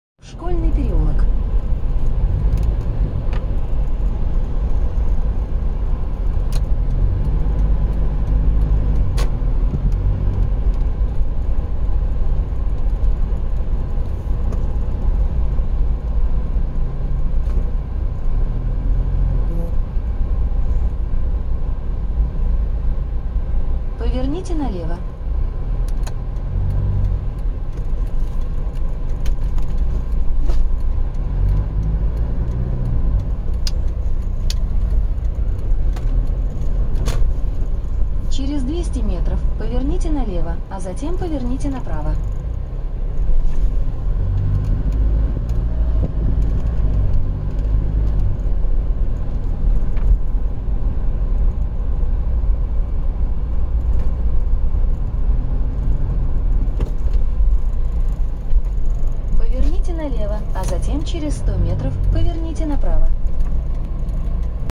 Навигатор -в пути -часть -вторая.
Путешествие с Навигатором, по незнакомым дорогам города.